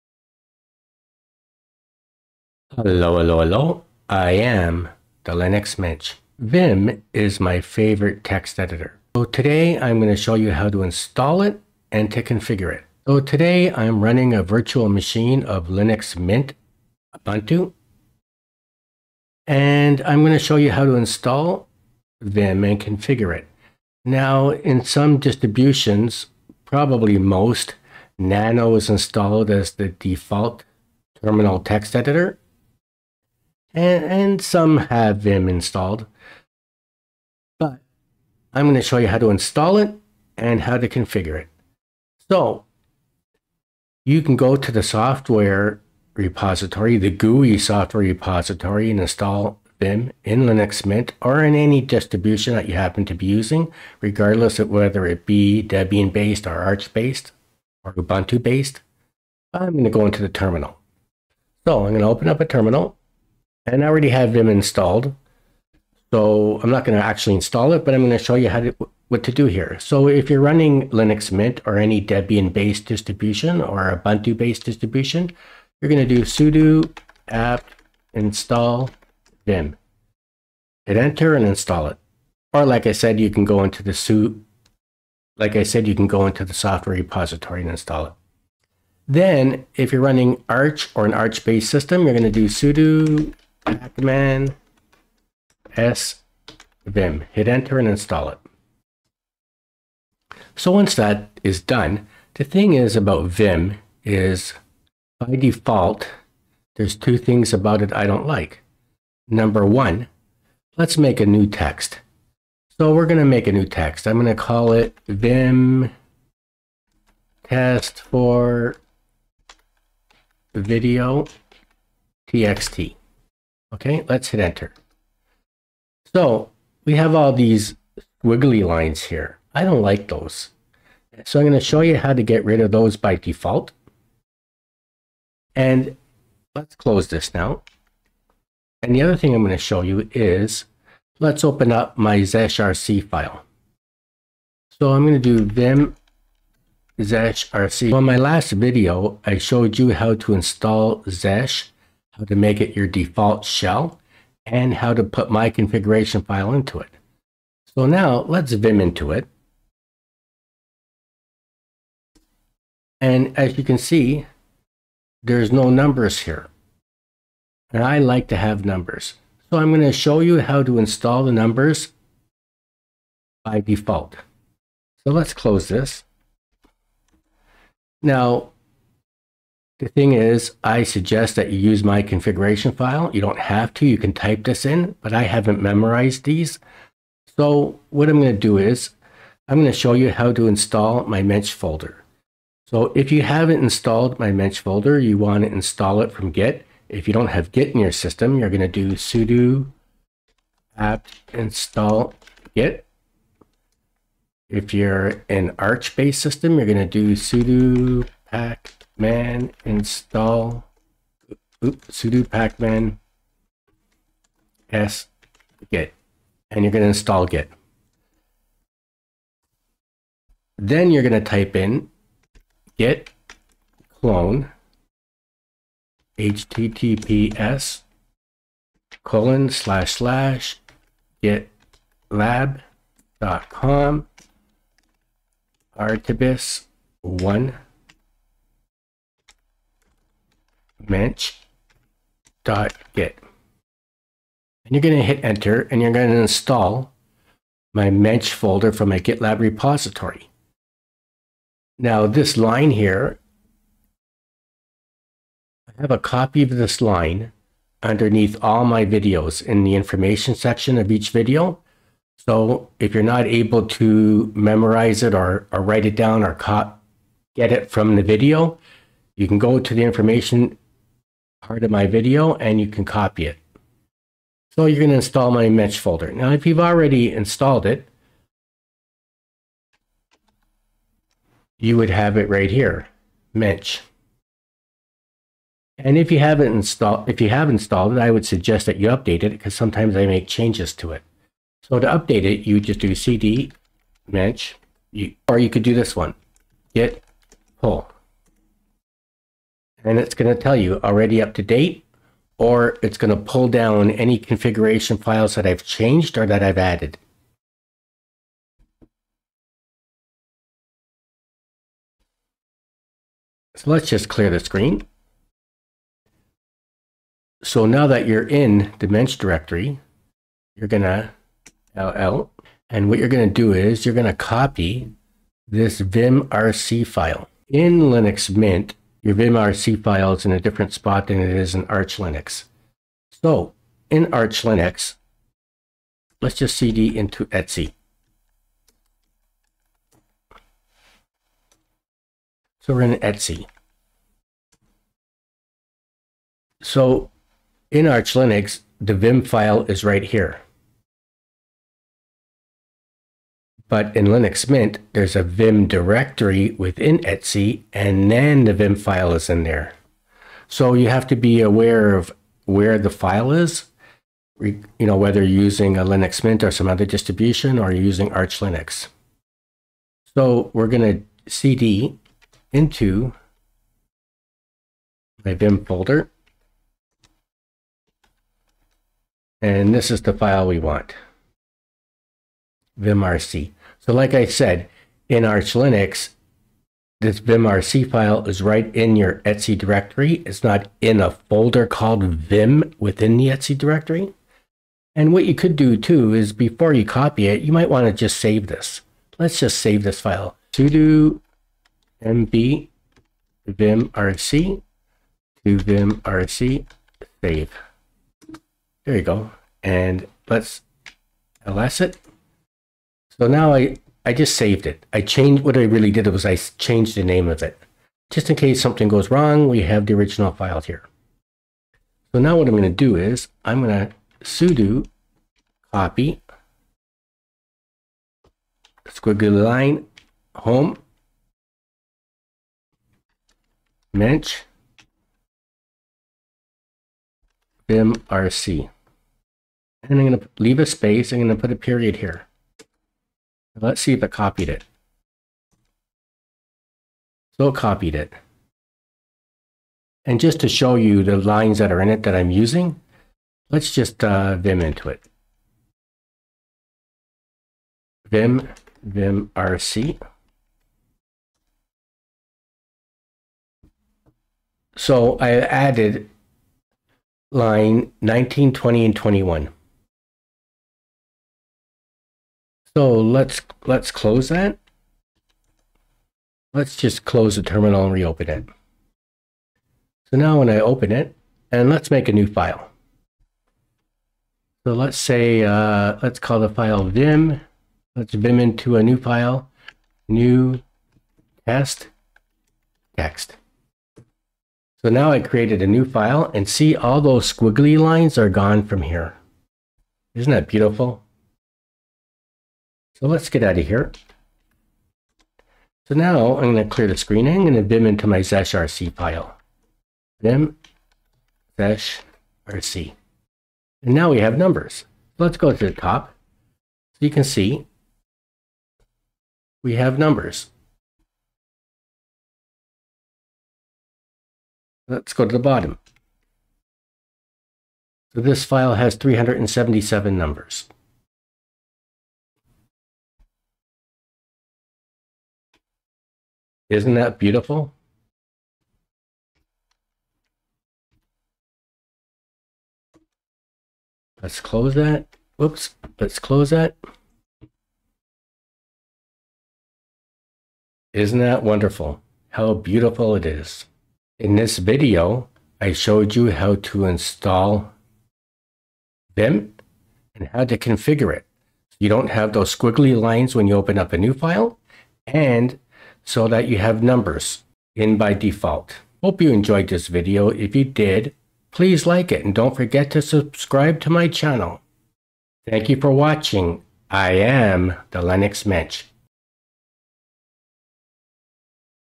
Vim Install & Configure
In this video, I show all to install Vim, configure it with numbers, and with no ~ lines in new text, in Linux Mint